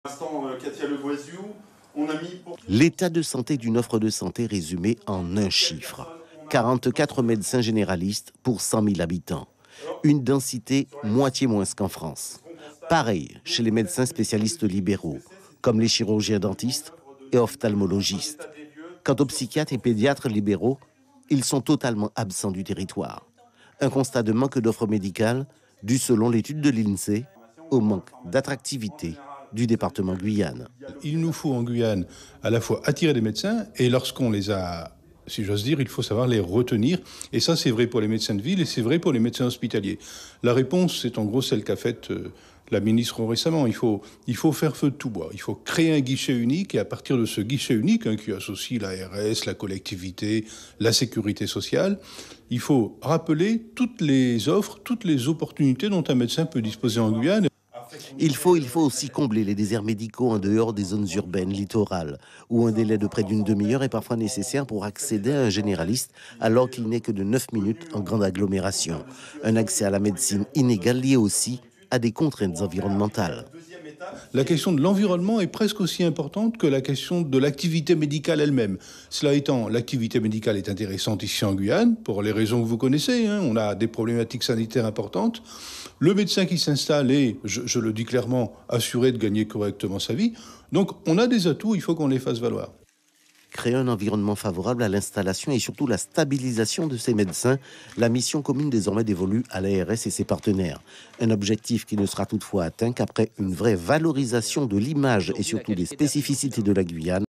Santé : un énorme déficit de médecins en Guyane
Santé : un énorme déficit de médecins en Guyane. En cause l'image dégradée de laGuyane qui n'attirent pas les professionnels de santé.